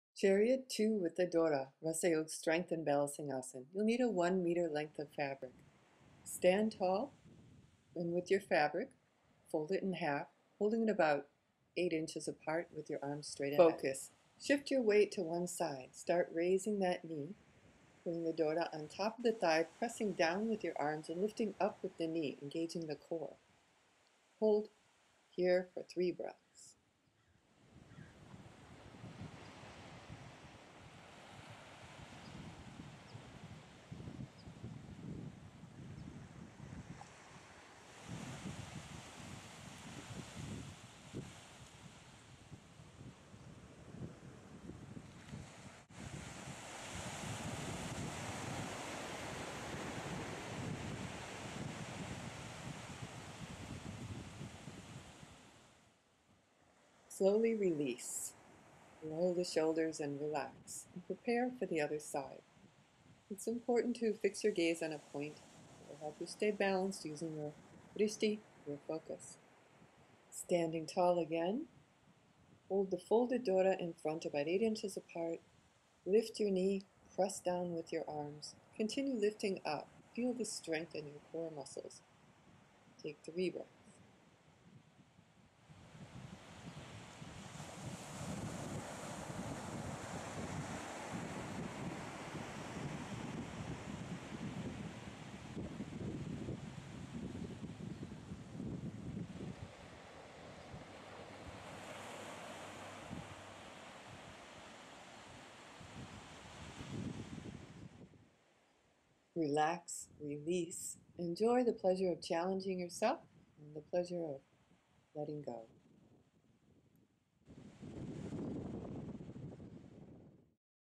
Firm the Triceps and balance: RussaYog Chariot 2 Asan
RussaYog® Chariot 2 posture strengthens the triceps while improving balance and core strength. Use a one-meter length of fabric with this posture.